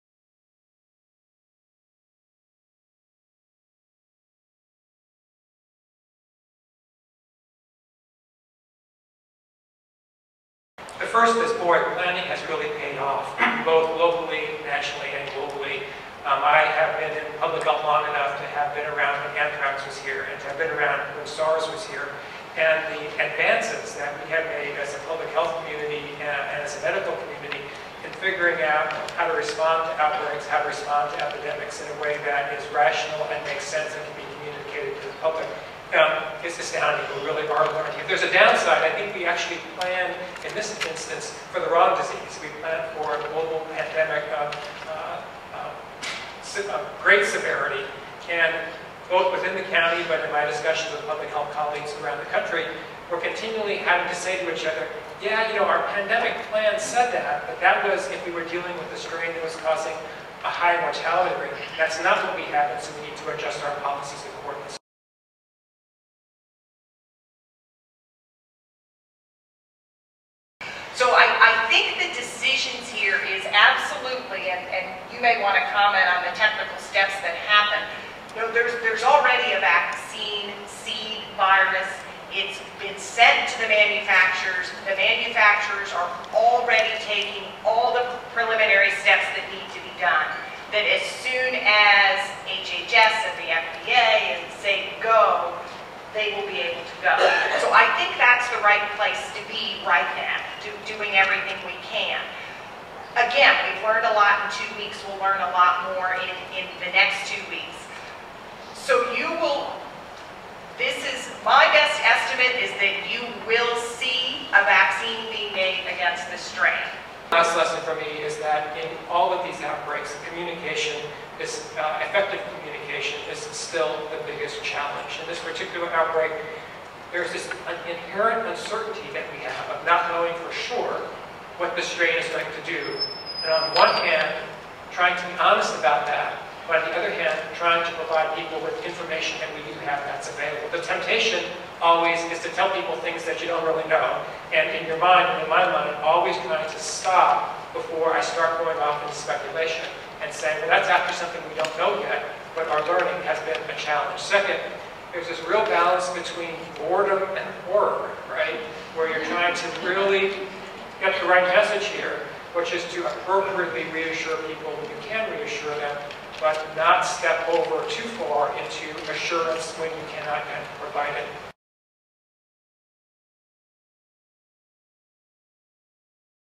Swine flu panel
Health professionals discuss swine flu during a May 4 meeting of the Washington Global Health Alliance at the University of Washington.